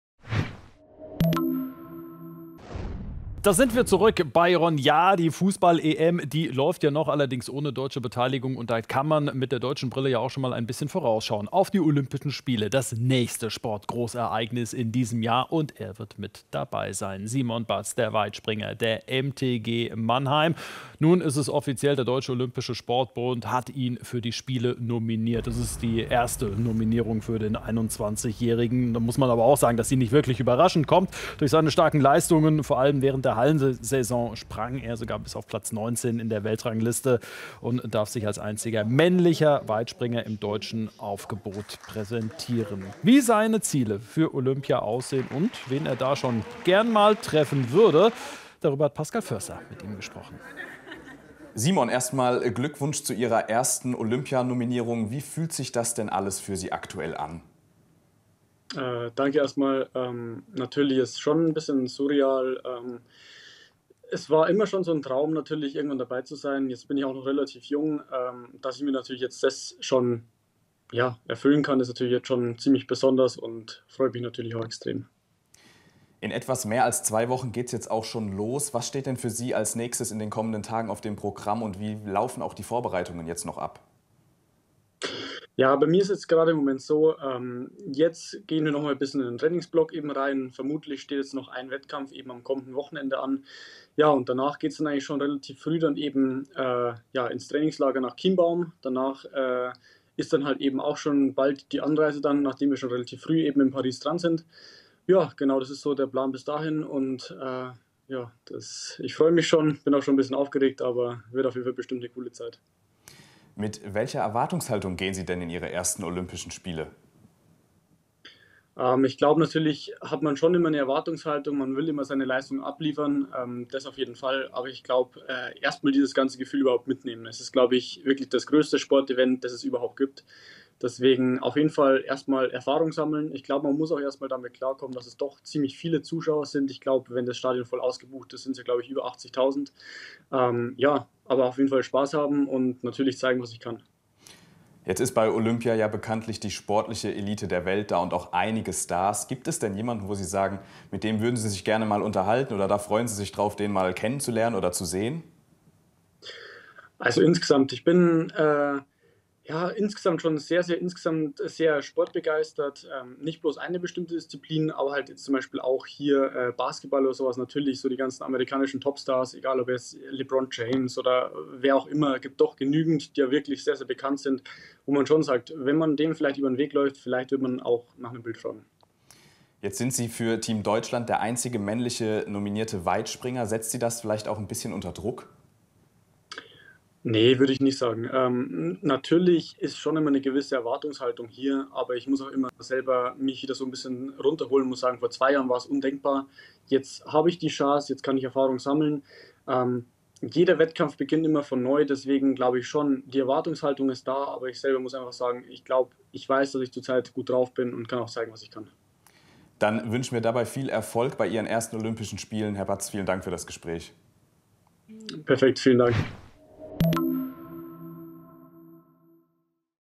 Simon Batz für Olympia nominiert | RON TV
Letzter Schritt für Olympia: Nur noch etwas mehr als zwei Wochen, dann starten die Olympischen Spiele in Paris. Neben der Fußball-EM das Sportgroßereignis des Jahres, bei der auch einige Sportler aus der RON-Region dabei sein werden. Unter anderem wird auch MTG Mannheim Weitspringer Simon Batz in die französische Hauptstadt fahren, das gab der Deutsche olympische Sportbund jetzt bekannt. Wir haben mit ihm über seine Ziele bei Olympia gesprochen.